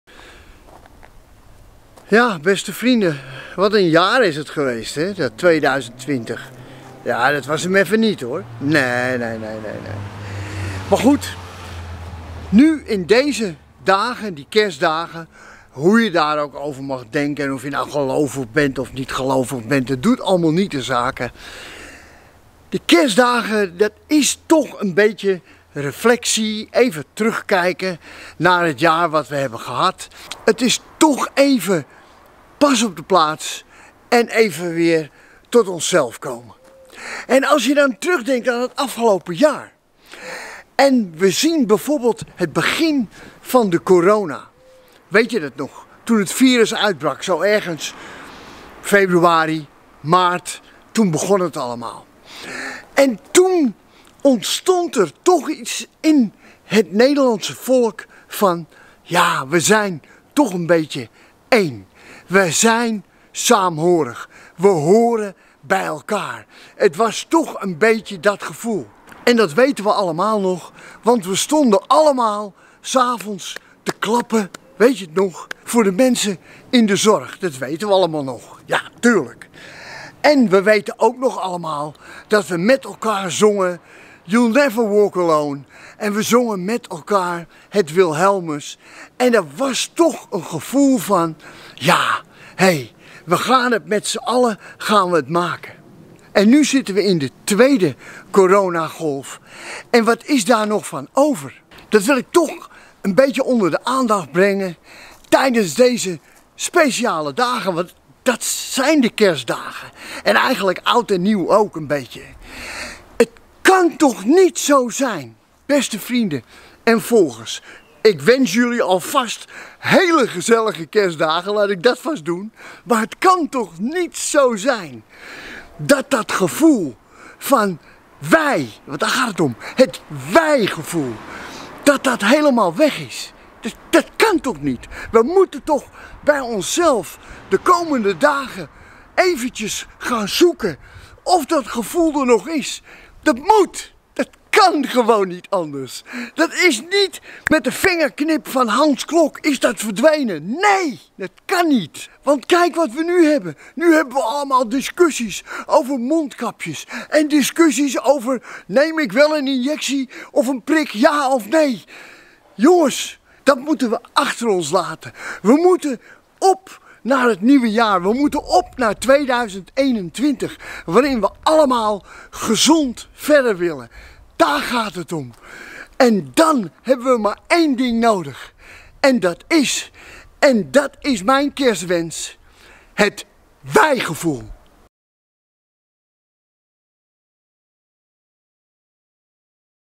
Harry's kersttoespraak!
Harry heeft maar één kerstwens! En dat is: dat ons WIJ-gevoel weer terugkomt. Harry wenst alle volgers ook namens Jut hele gezellige Kerstdagen!